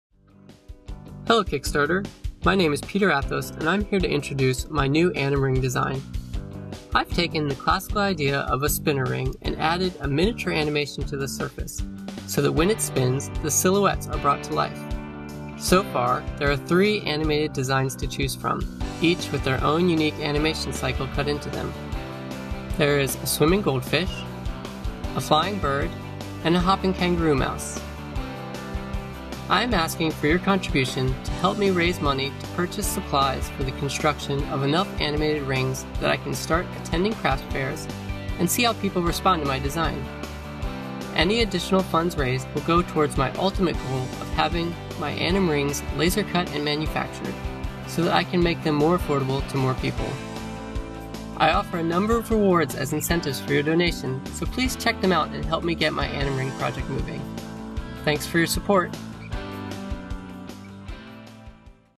AnimRings animation rings
This is a video introduction for my new AnimRing Kickstarter project featuring wearable ring designs that animate when they spin!